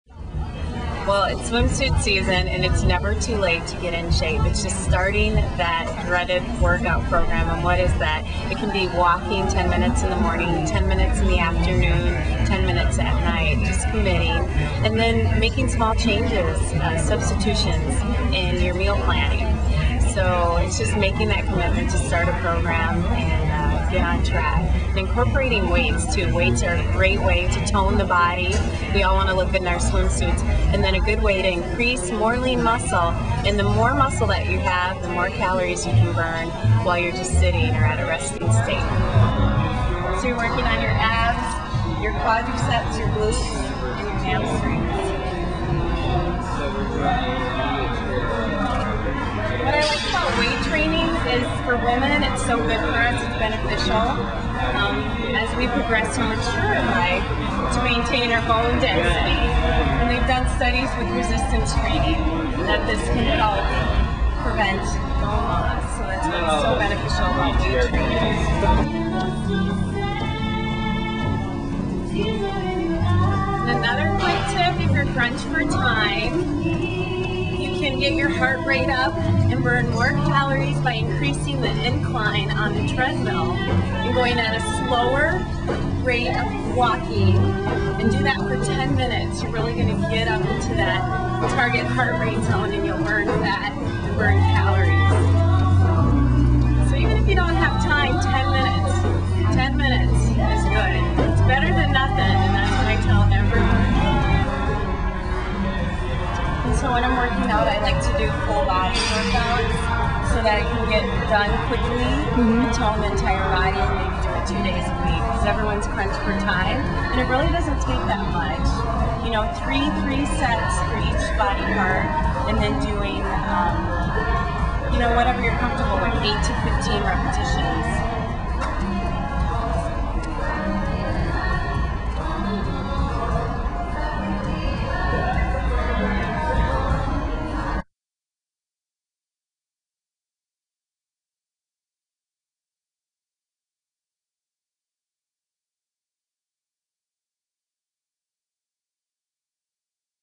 Ms. Bikini Universe 2005 shares her exercise secrets
Melissa Hall, Ms. Bikini Universe 2005, works out at Evolve Fitness in Framingham, Mass., and tells us how she stays in shape.